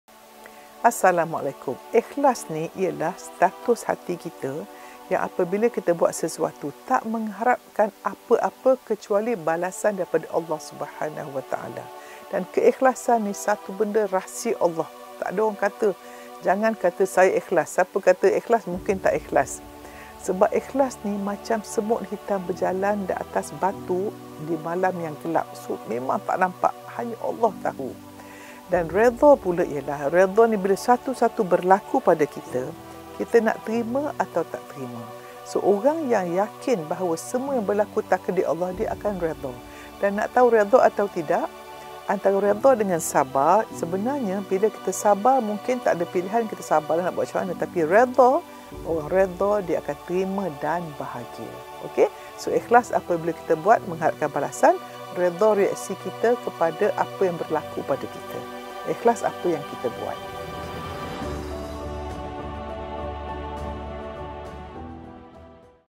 Perbezaan Ikhlas Dan Redha Dari Prof Muhaya
Apa yang anda faham dengan maksud ikhlas dan redha? Jom dengarkan penjelasan daripada Prof Muhaya.